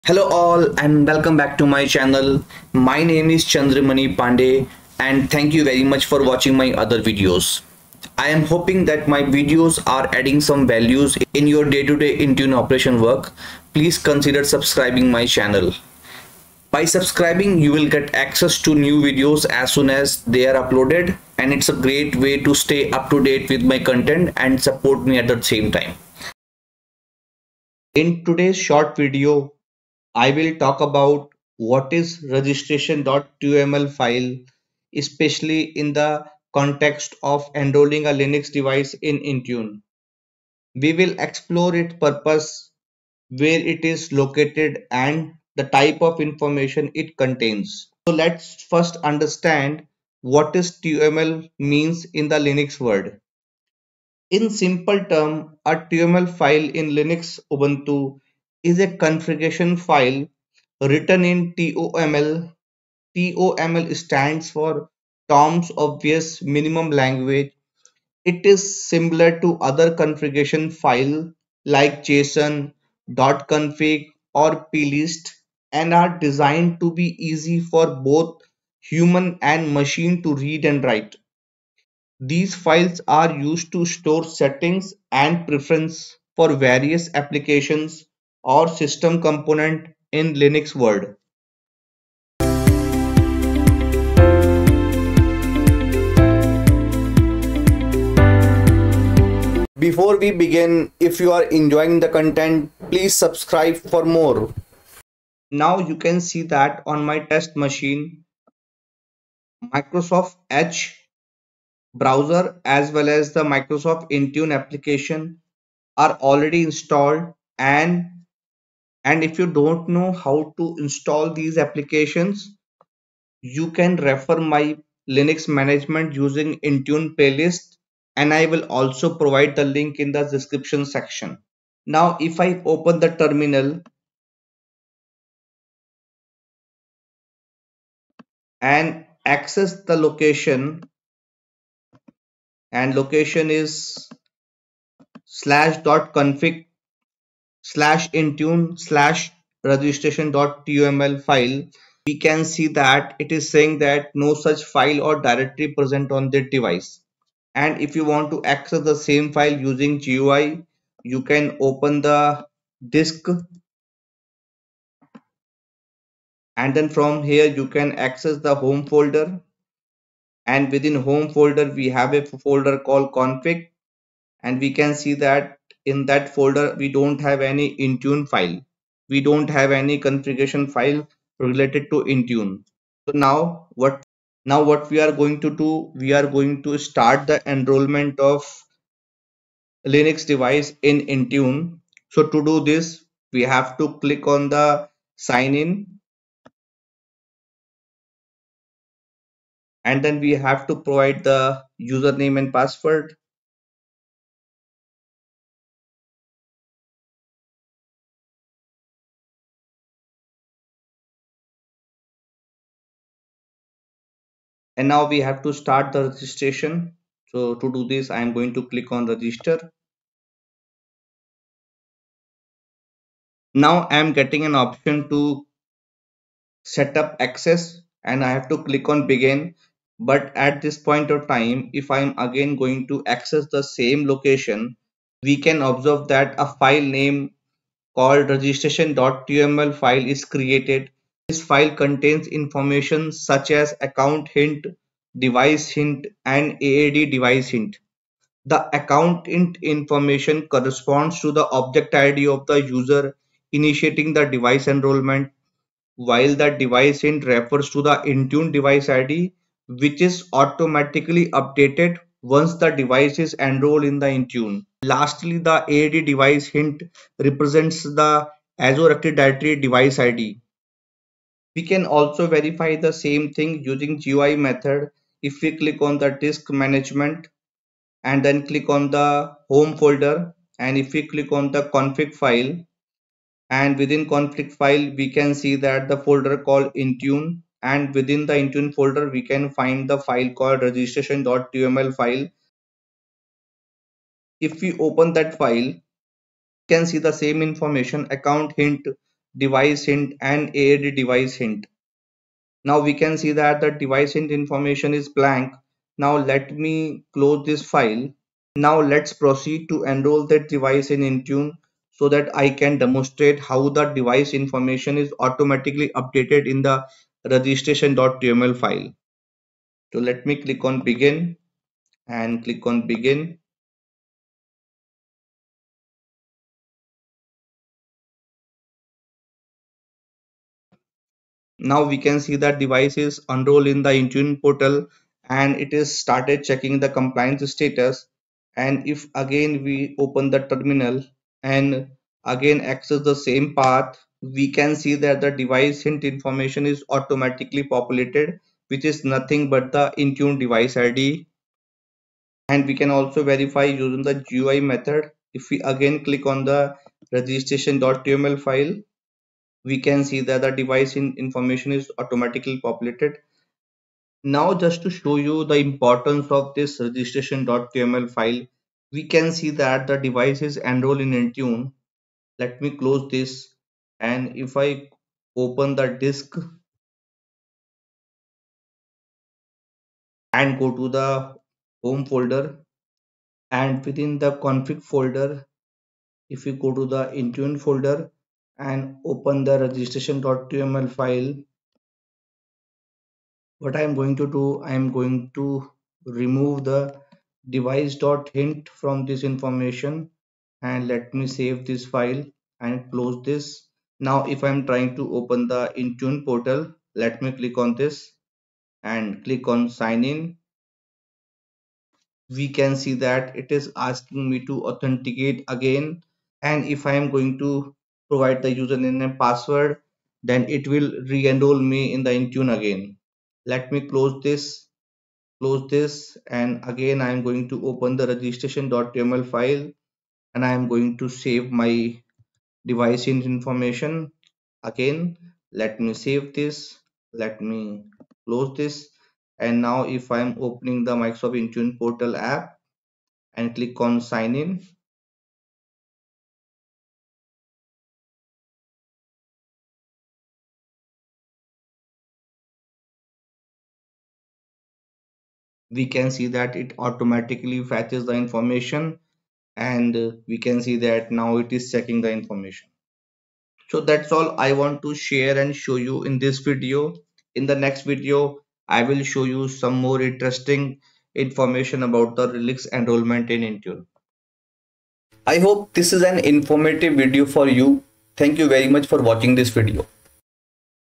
Enroll Linux into Intune troubleshooting - What is Registration.toml
📚 Enroll Linux into Intune - What is Registration.toml | Enroll Ubuntu in Intune | Intune troubleshooting

👉 In today's short video, I'll talk about what is registration.toml file, specifically in the context of enrolling a Linux device in Intune. We'll explore its purpose, where it's located, and the type of information it contains